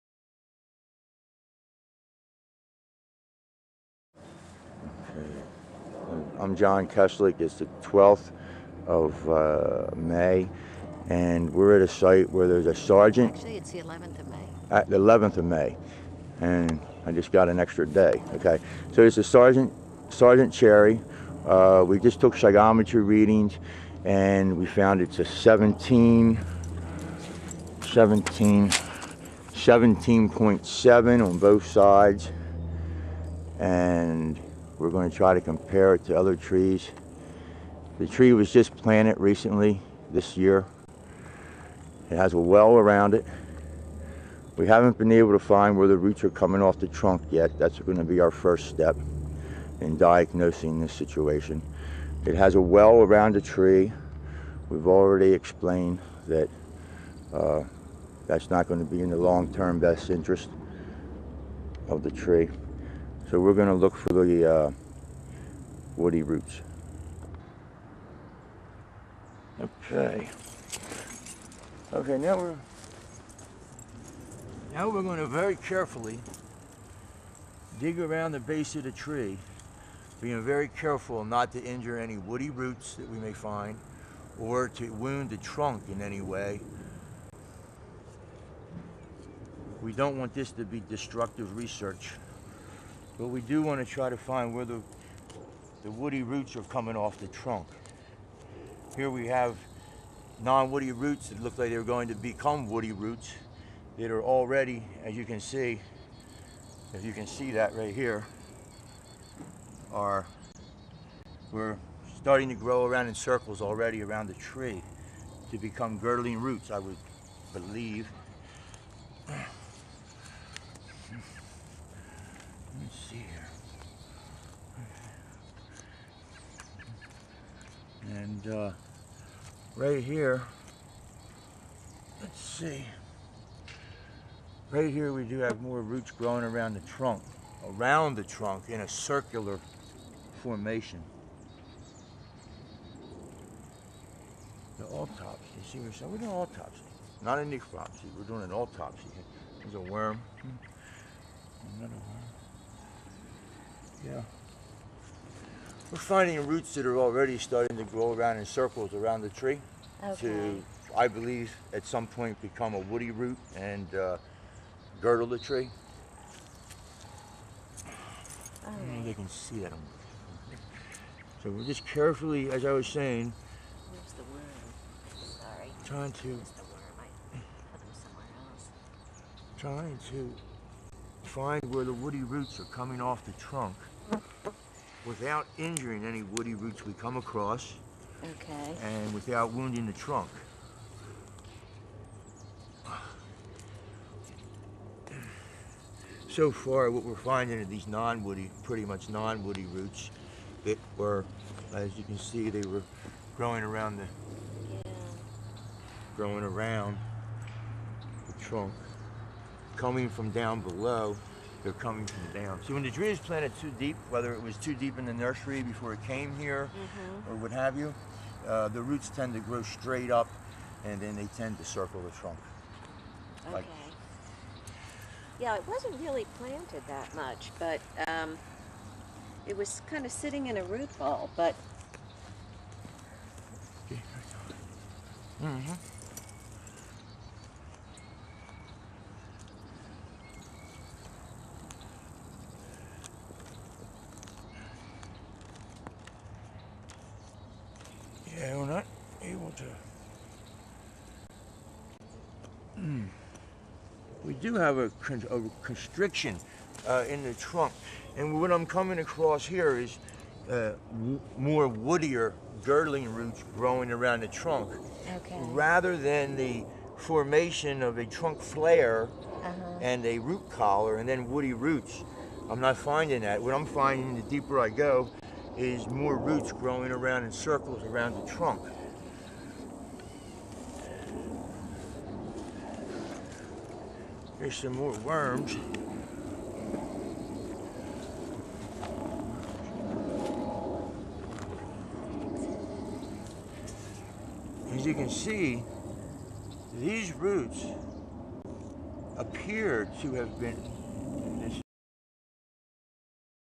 Not insects - tree planted too deep - 1 of 2
First order of diagnosis of a tree by a forest / tree consultant with a background in tree biology.  A trees vigor was designed to be planted at the depth the woody roots come off the trunk of the tree.  I.e., unless you are planting a young bare root trees, then at the depth the non-woody roots come off the tree.  I believe he vitality of a tree as well as their associates, greatly depends on the depth the tree is planted.  No one wins when a tree is planted too deep.  The predisposition set by deep planting does set the stage for insects and fungi as well as bacteria that too often get the blame the entire low quality life of the tree.  There are two parts.  They can be found at our dictionary under "video's".